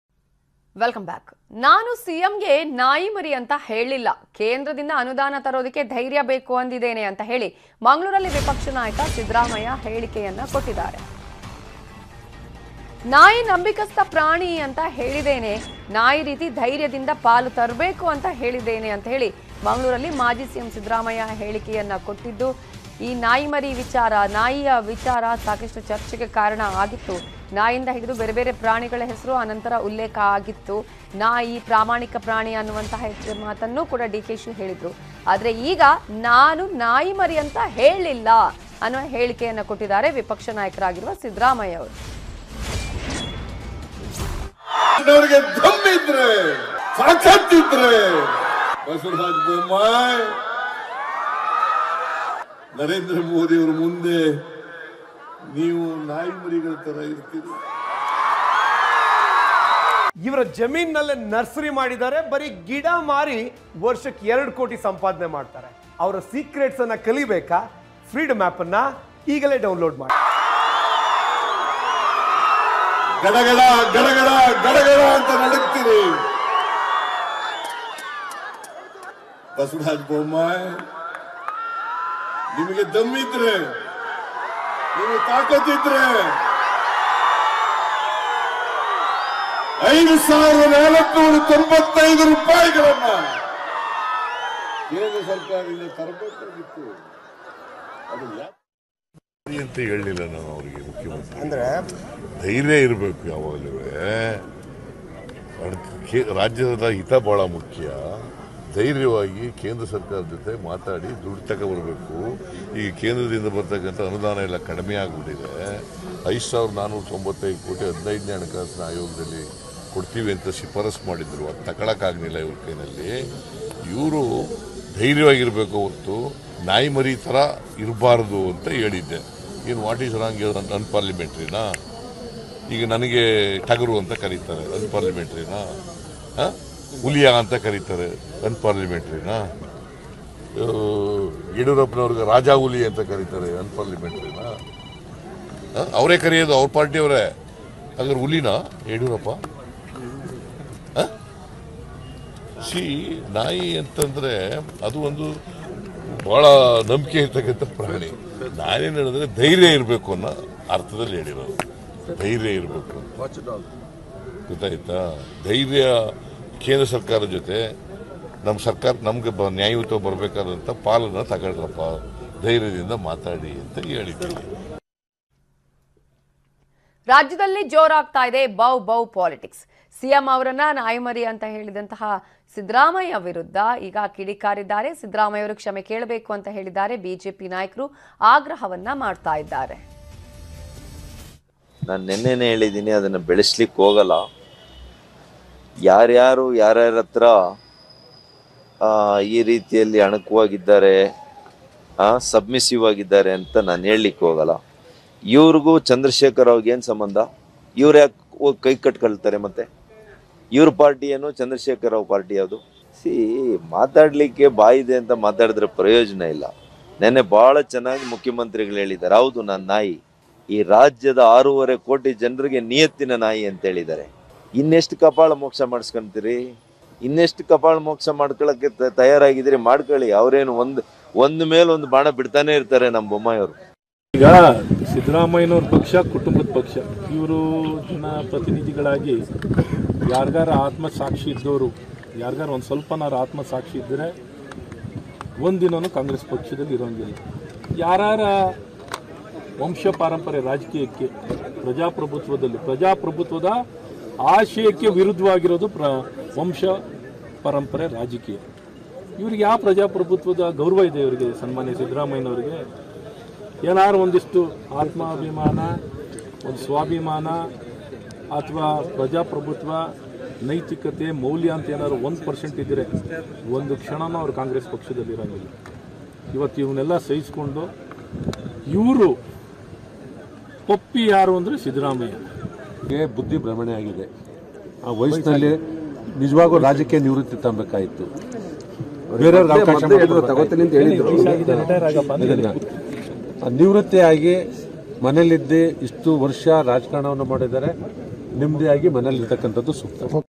Siddaramaiah | ನಾನು ನಾಯಿ ಎಂದು ಸಿಎಂಗೆ ಹೇಳೇಯಿಲ್ಲ ಎಂದು ಉಲ್ಟಾ ಹೊಡೆದ ವಿಪಕ್ಷ ನಾಯಕ ಸಿದ್ದರಾಮಯ್ಯ | Cm Bommai
ಕೇಂದ್ರದಿಂದ ಅನುದಾನ ತರಲು ಧೈರ್ಯ ಬೇಕೆಂದು ಹೇಳಿದ್ದೇನೆ. ಸಿಎಂ ನಾಯಿ ಮರಿ ನಂಬಿಕಸ್ಥ ಪ್ರಾಣಿ ಎಂದಿದ್ದಾನೆ ಎಂದು ಮಾಜಿ ಸಿಎಂ ಸಿದ್ದರಾಮಯ್ಯ ಮಂಗಳೂರಿನಲ್ಲಿ ಹೇಳಿಕೆ ಕೊಟ್ಟಿದ್ದಾರೆ.